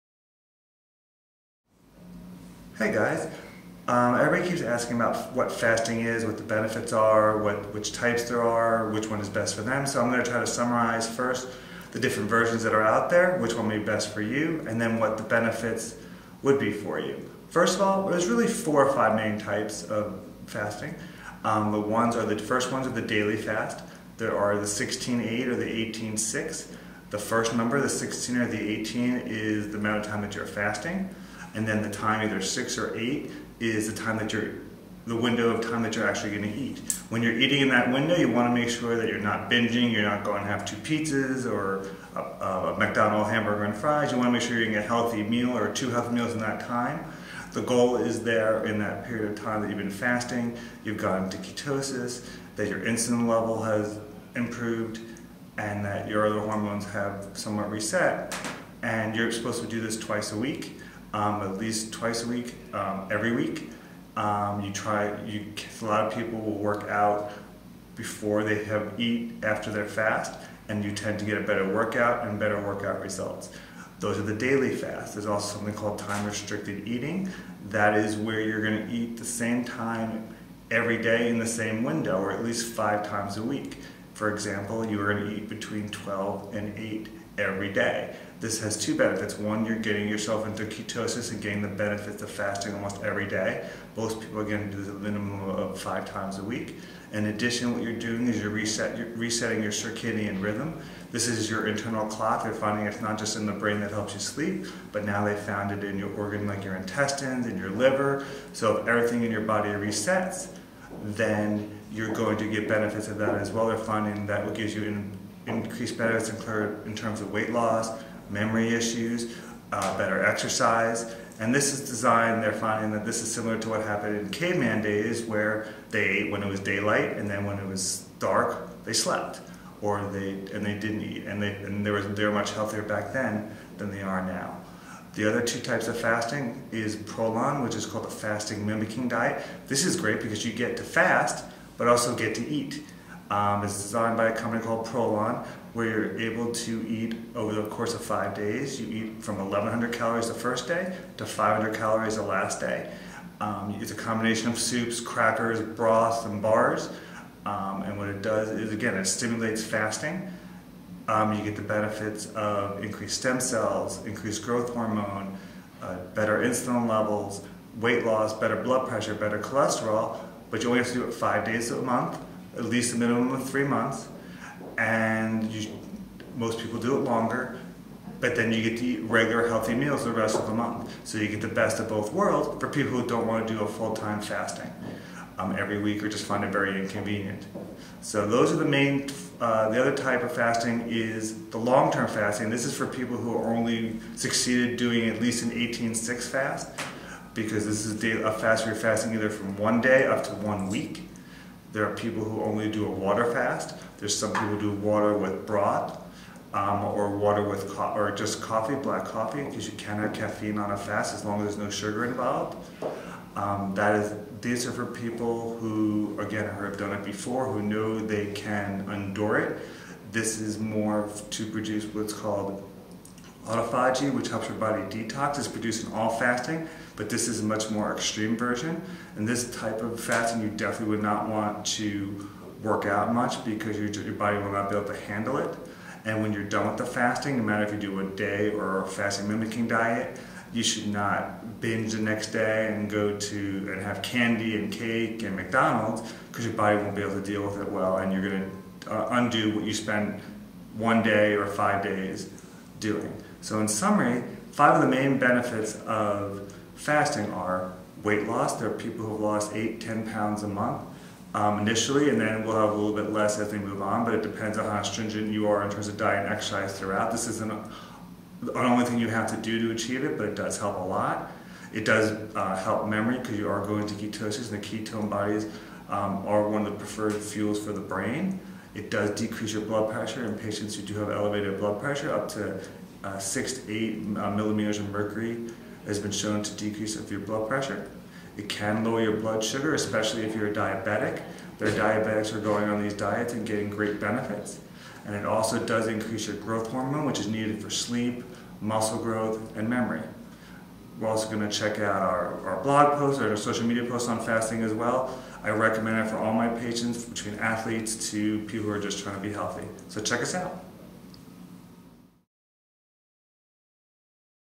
Fasting
These are the different types of fasting, and the benefits you get from them.